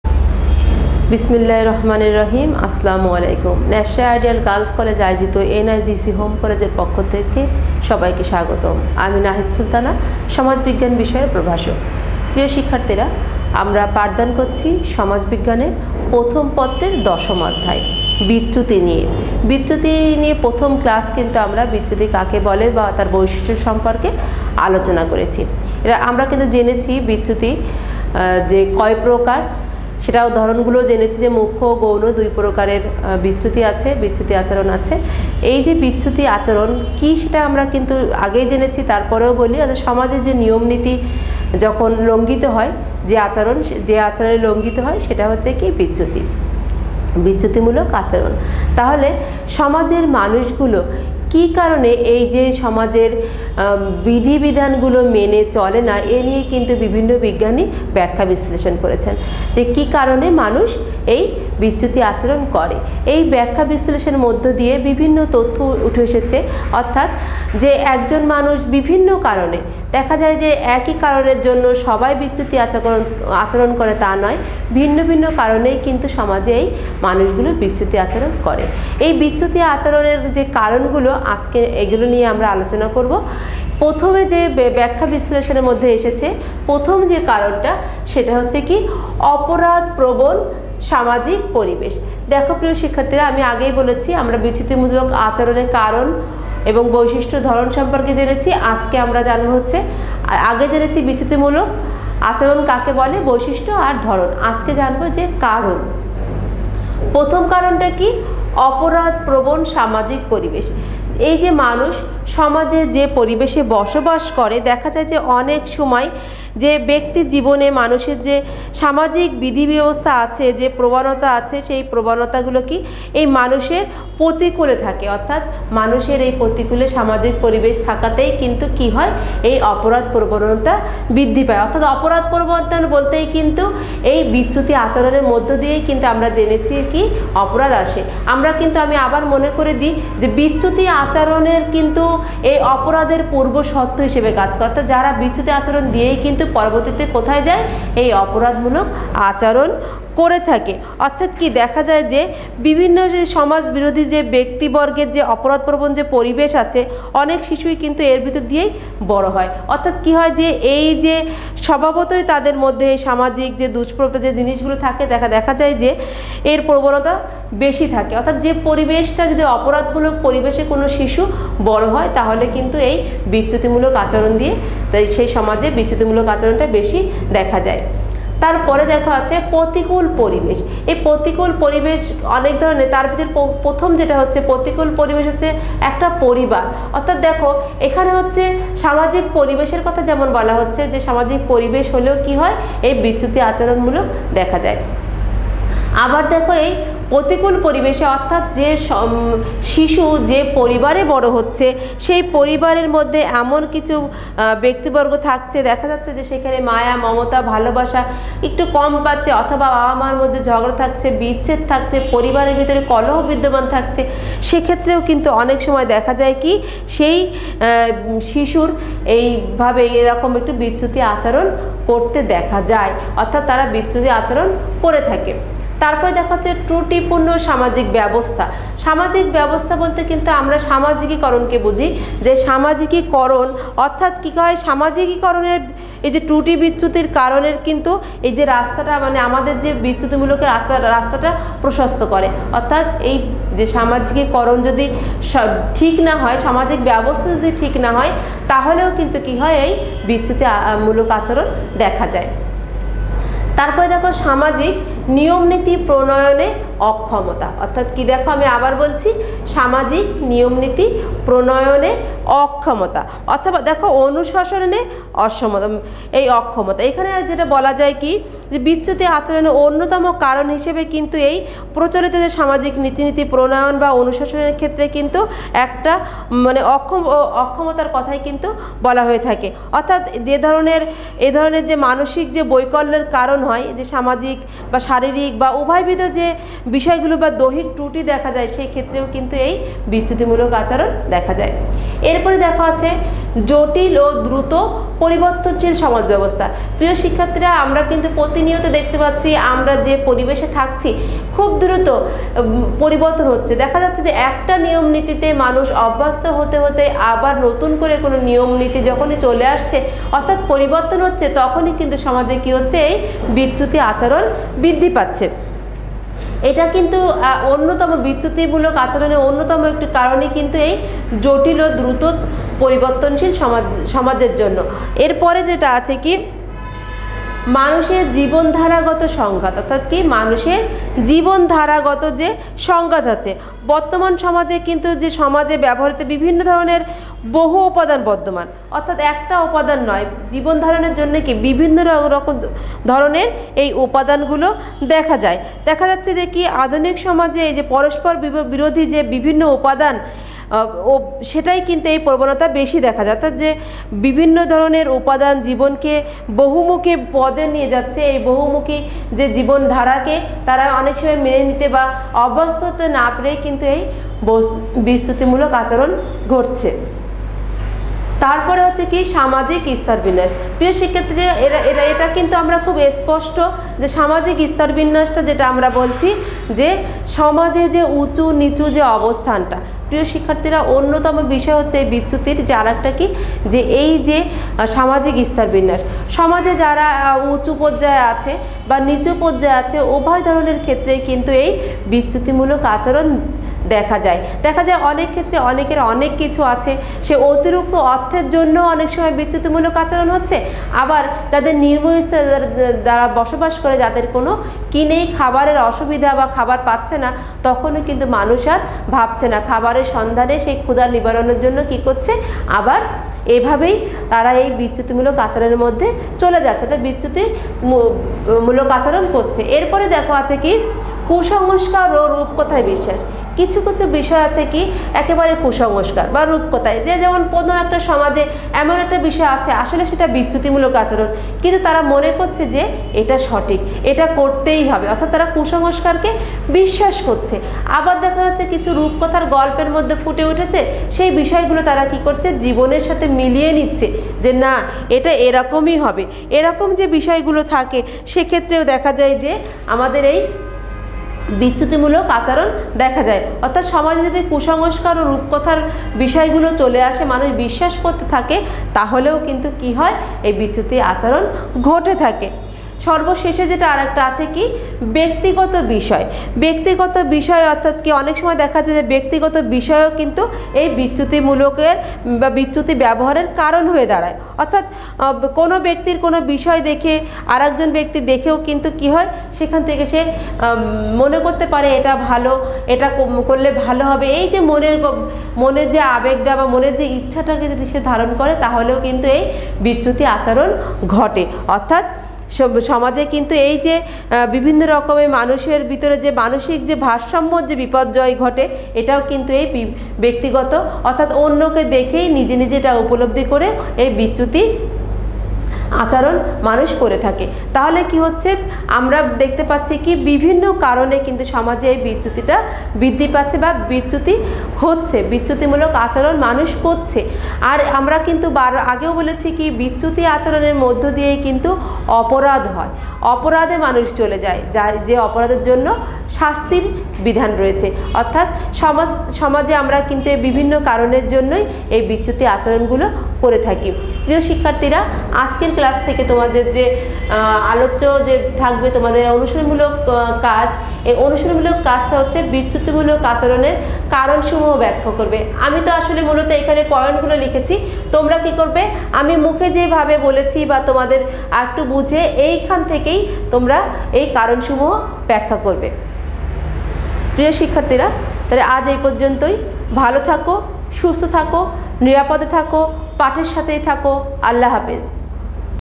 দ্বাদশ (৪র্থ পর্ব) শ্রেণির সমাজবিজ্ঞান, লেকচার নং-২, আলোচ্য বিষয়: বিচ্যুতিমূলক আচরণের কারণ
শ্রেণি: দ্বাদশ (৪র্থ পর্ব)
বিষয়: সমাজবিজ্ঞান
লেকচার নং-২
আলোচ্য বিষয়: বিচ্যুতিমূলক আচরণের কারণ
প্রভাষক: জ. নাহিদ সুলতানা